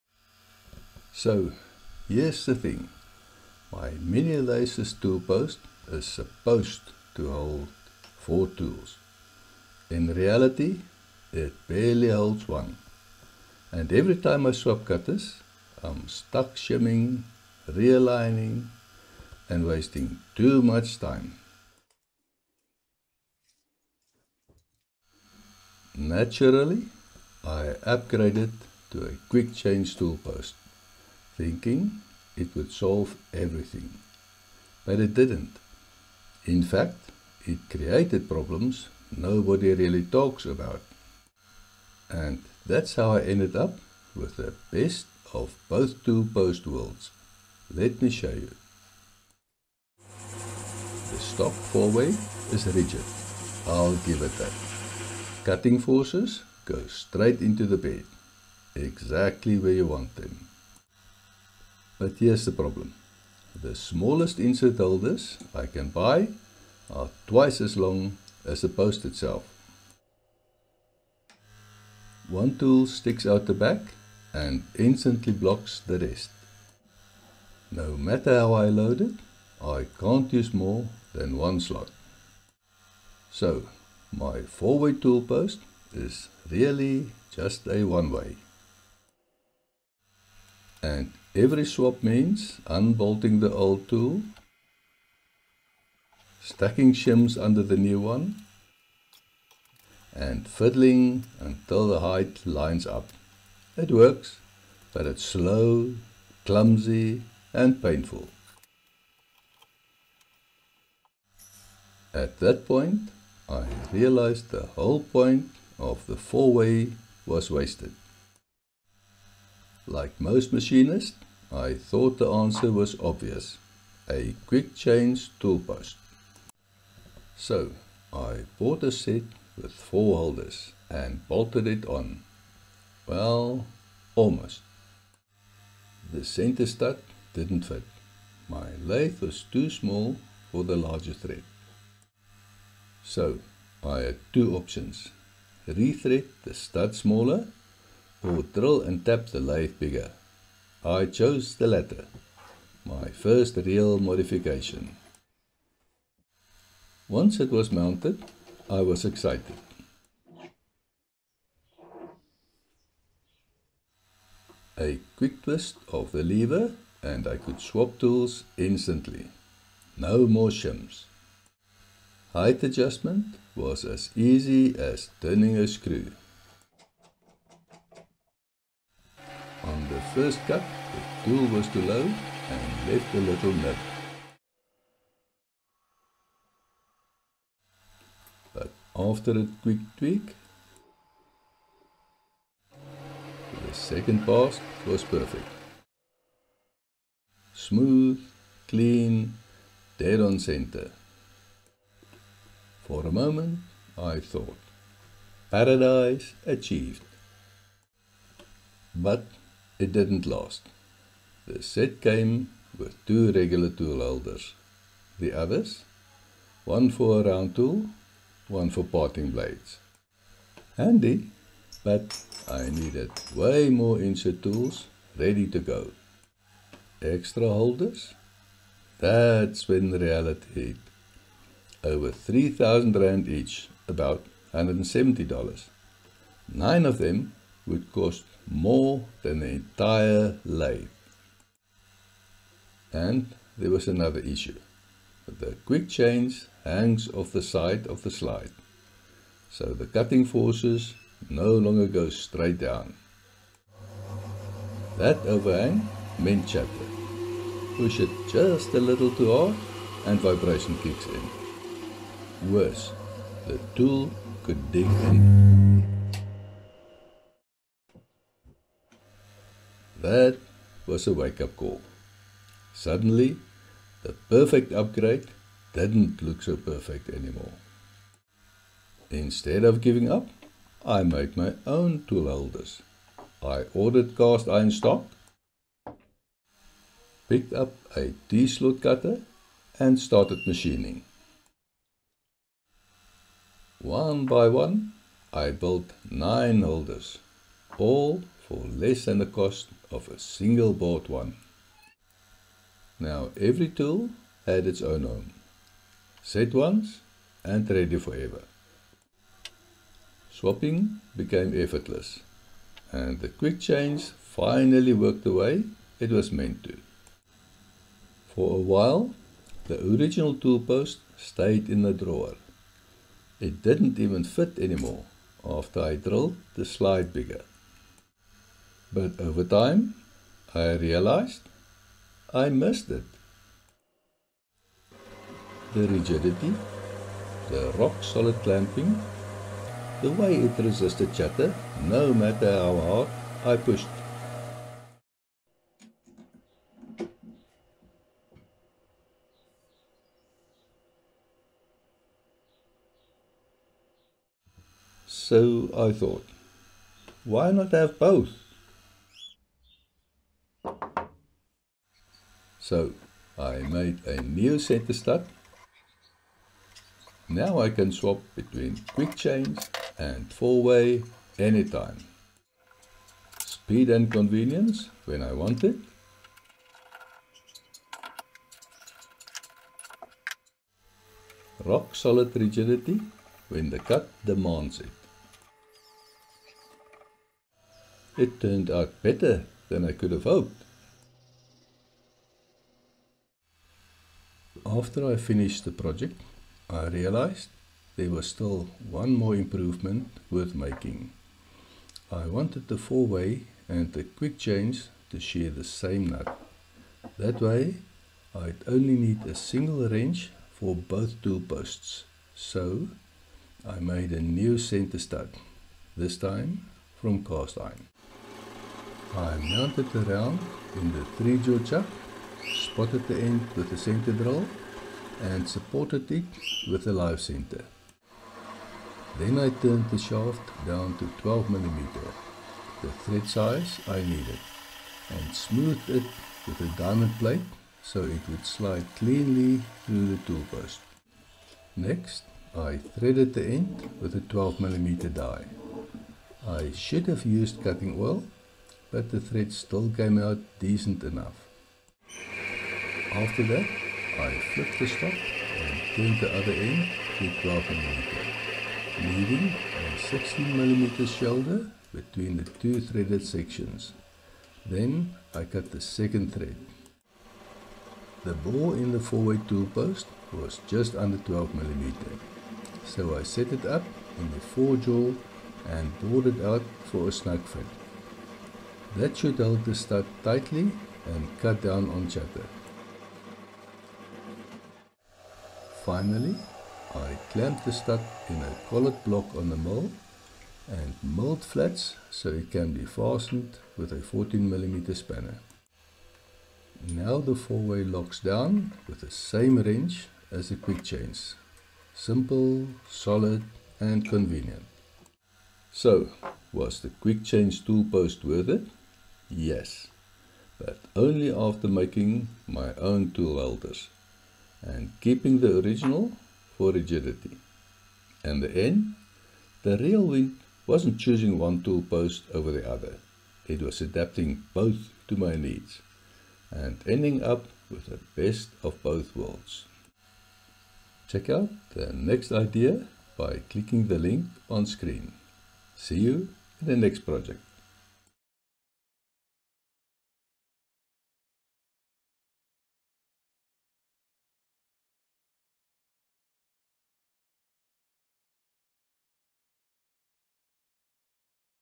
4 Way vs Quick Change Toolpost Best of Both Worlds
My mini lathe’s toolpost has always been a love-hate relationship. The stock 4-way is rock-solid but clumsy, while the quick-change is fast but expensive and less rigid. So instead of choosing one, I adapted both. Ending up with the best of both worlds.
In this video, I show the full journey:
• Why the stock 4-way is really just a “1-way”
• The hidden problems with a quick-change toolpost
• How I made my own tool holders from cast iron
• And finally, how I combined both systems with a custom centre stud
Bonus Update: After finishing the project, I realised I could simplify things even more. I made a brand-new centre bolt from cast iron so that both the quick-change and the 4-way use the same nut, meaning just one wrench for both toolposts. That little detail makes toolpost swaps faster, neater, and more convenient.
In the end, it wasn’t about choosing one toolpost over the other. It was about adapting both to my needs.

Timestamps:
00:00 Introduction
00:50 The Problem with the Stock Toolpost
01:59 The Dream Solution: Quick-Change Toolpost
03:28 The Hidden Problems
04:38 The DIY Fix: Making My Own Holders
05:17 The Final Twist: Best of Both Worlds
06:33 Add-On: Making the Cast Iron Centre Bolt
08:25 Outro